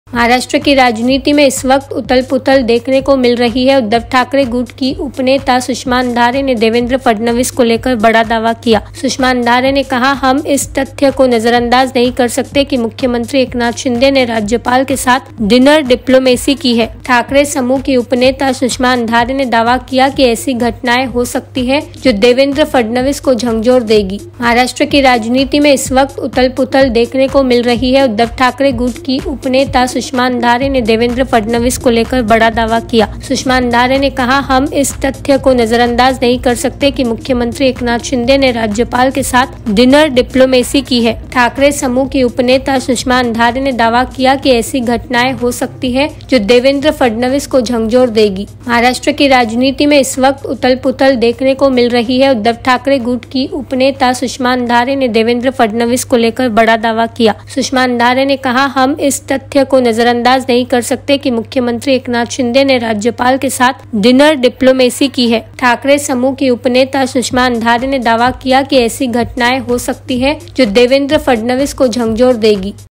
Devendra Fadnavis के दावे पर सुषमा अंधारे का पलटवार
महाराष्ट्र की राजनीति में इस वक्त उथल-पुथल देखने को मिल रही है. उद्धव ठाकरे गुट की उप नेता सुषमा अंधारे ने देवेंद्र फडणवीस को लेकर बड़ा दावा किया है. सुषमा अंधारे ने कहा, हम इस तथ्य को नजरअंदाज नहीं कर सकते कि मुख्यमंत्री एकनाथ शिंदे ने राज्यपाल के साथ डिनर डिप्लोमेसी की है. ठाकरे समूह की उप नेता सुषमा अंधारे ने दावा किया है कि ऐसी घटनाएं हो सकती हैं जो देवेंद्र फडणवीस को झकझोर देंगी.